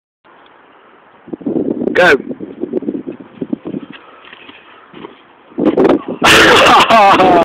Sams Amazin Flip Onto Trampy Mattress
Sam doin a flip on to this really trampy mattress in park an lands on his back lol